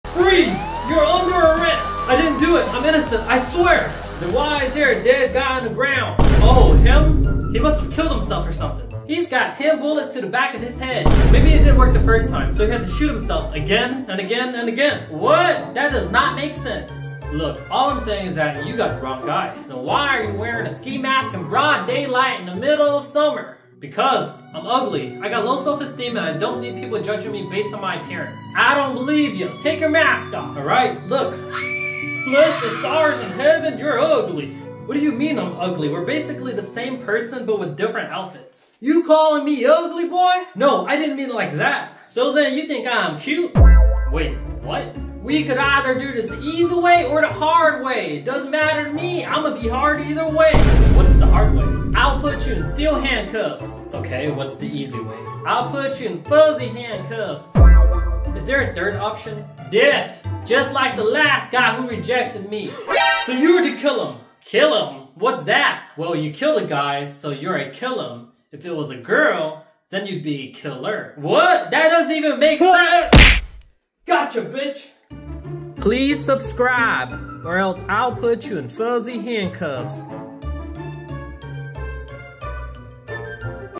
When you're Innocent but look GUILTY AF 🤣😭
What if you looked guilty but you're innocent? This is a funny skit video about a guy who looks guilty but is actually innocent. The real criminal will surprise you... skit video by Lime Tuul.